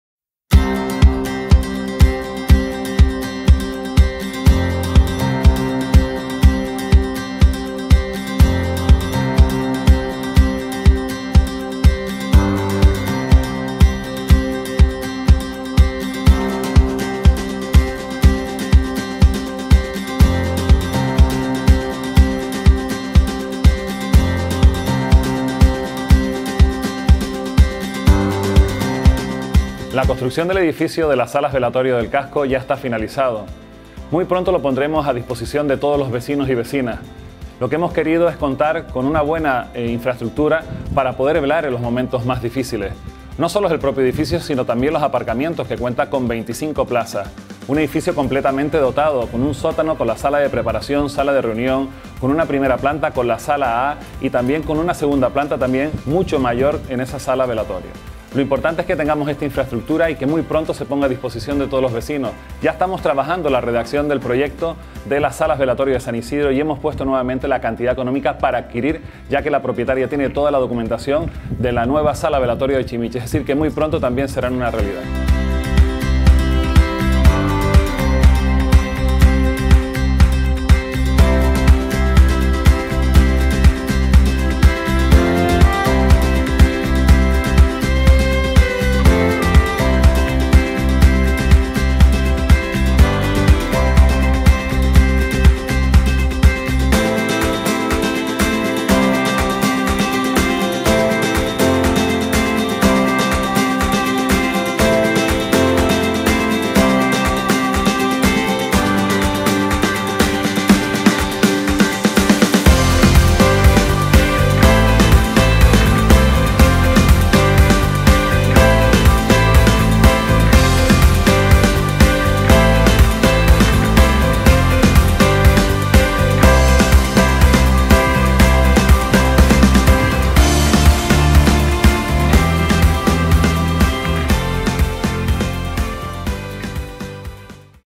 Salas Velatorio de Granadilla casco
El Ayuntamiento culmina la construcción del edificio de las salas velatorio en Granadilla casco; un recurso, que bajo el nombre de Nuestra Señora del Rosario.